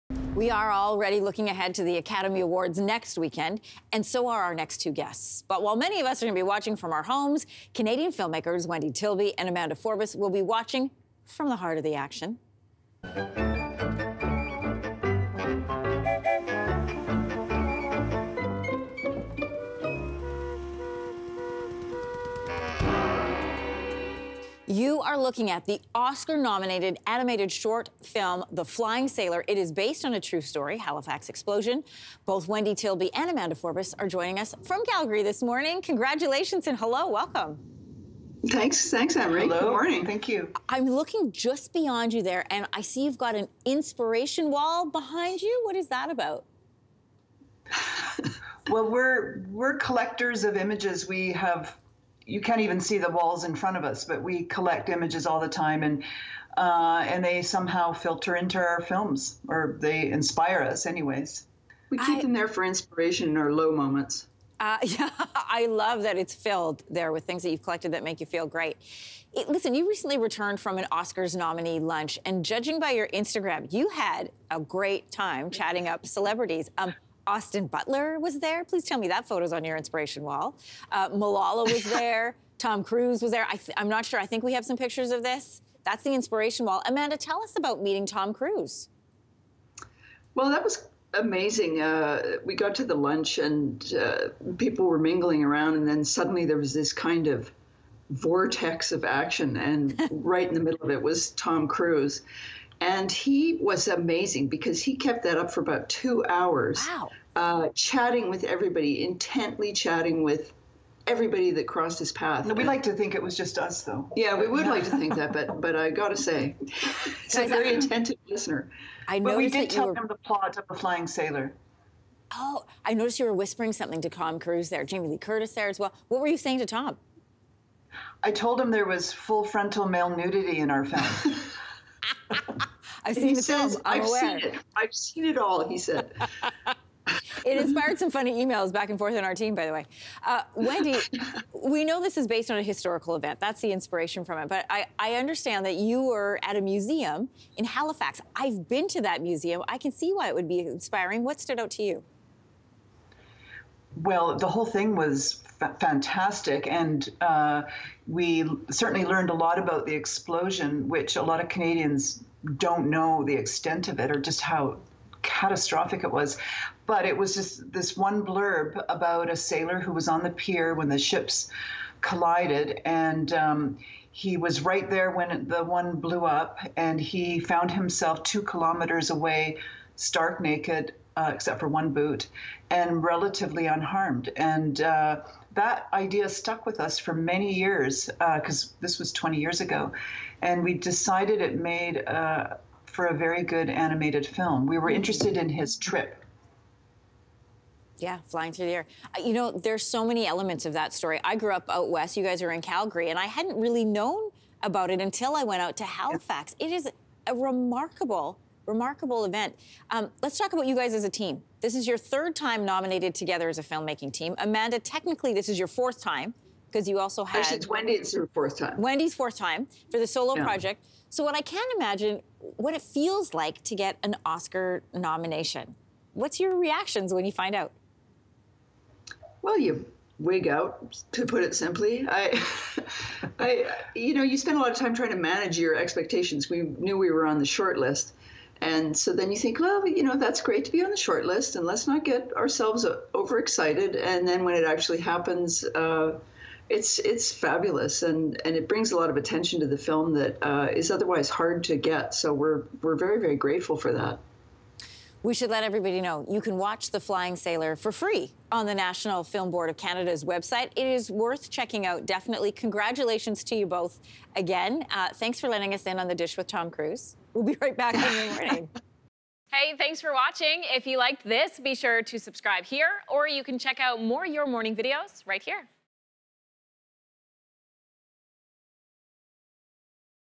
Canadian filmmakers heading to the Oscars | Your Morning
Wendy Tilby and Amanda Forbis are attending the Academy Awards for their nominated film ‘The Flying Sailor’.